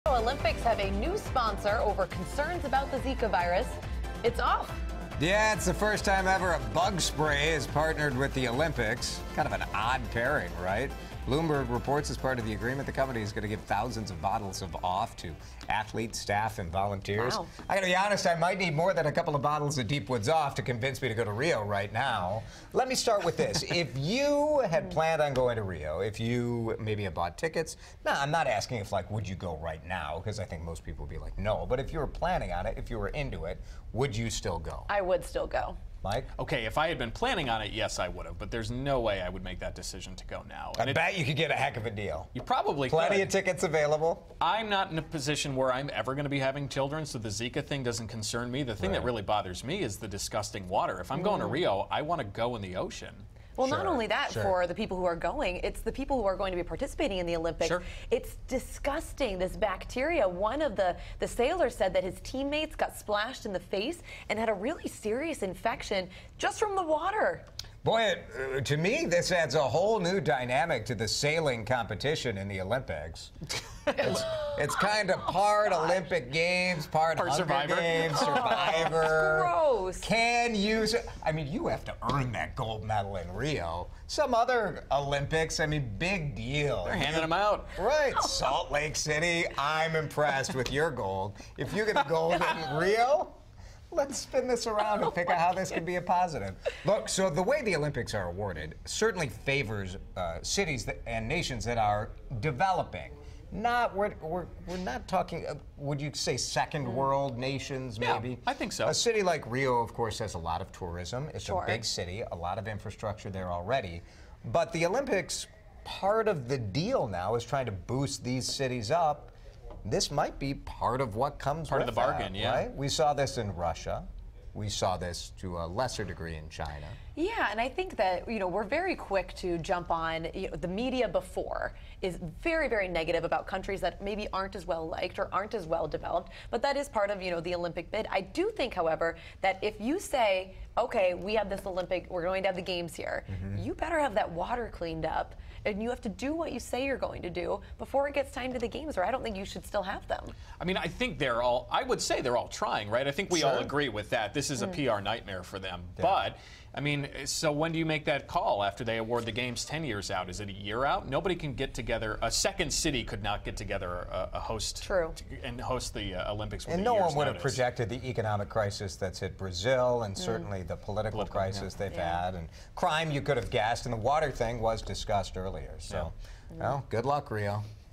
OFF! Bug Repellent Becomes Sponsor Of Rio Olympics
The bug repellent has partnered with Olympic games amidst concerns over Zika virus, Jason DeRusha, Kylie Bearse and Mike Augustyniak report (2:57). WCCO Mid-Morning – July 6, 2016